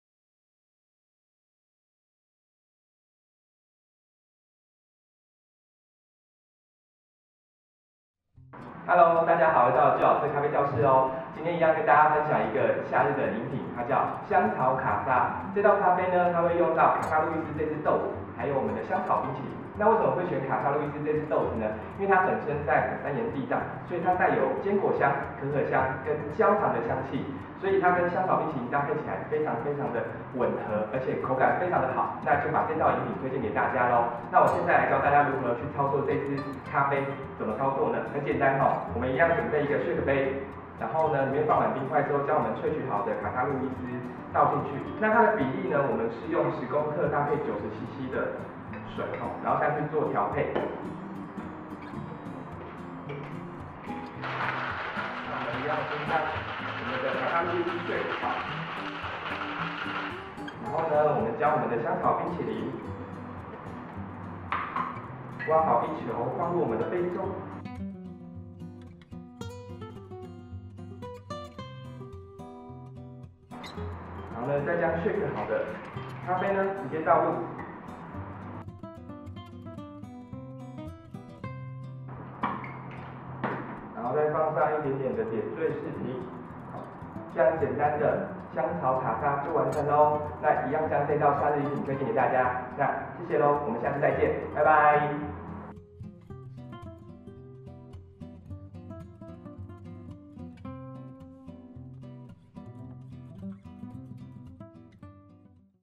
Driver《Joe ♥ 咖啡》香草卡莎
卡莎・露易斯帶有核桃香以及猶如麥芽糖般的清甜，尾韻藏著榛果可可的滑口，搭配濃郁芬芳的香草冰淇淋，毫無違和，絕對是炎夏消暑的女神級飲品！

冰咖啡可選擇濃度較高的手沖咖啡再進行冰鎮，也可使用帶有酒香的冰滴咖啡搭配冰淇淋，兩種截然不同的風味推薦給愛咖啡的你。😁